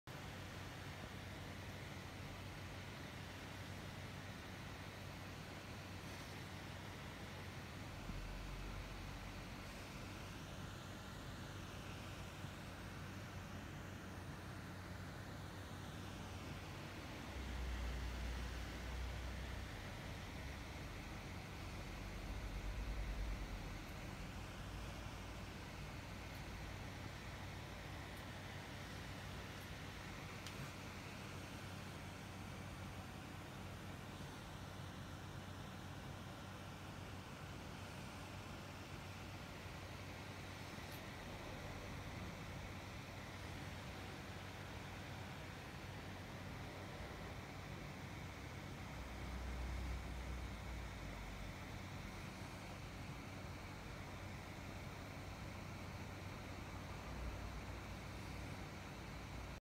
Happy Sleeping in the Closet クローゼットで眠るハッピー
Good evening!
Happy slept in the closet. Once she showed her face, but she hid it with her legs soon. Sleeping is important for her health. I hope she’ll live happily and healthily forever.
こんばんは
ハッピーがクローゼットで眠りました。一旦顔を見せましたが、すぐに足で隠しました。寝る事は健康にとって重要です。ずっと幸せで健康で生きられますように。